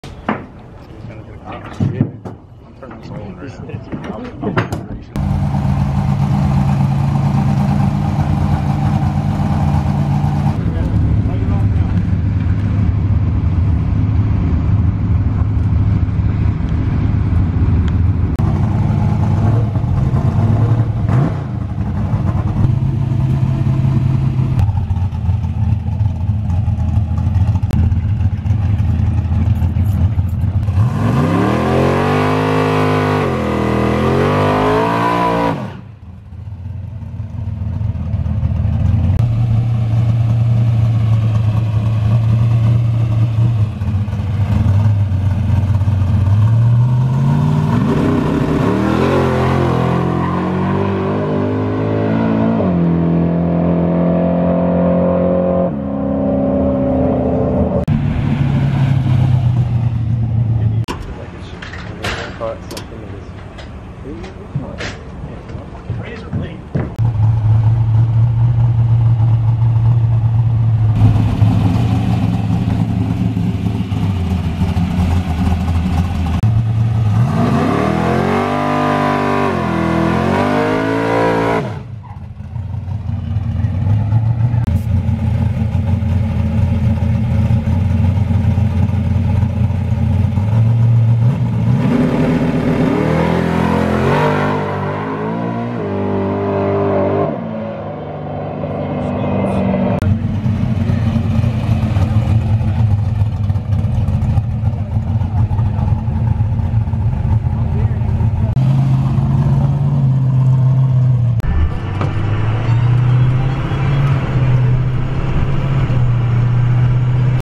Philly Test Hits (S10 Does Wheelie on the Street)
private road, professional driver, bla bla bla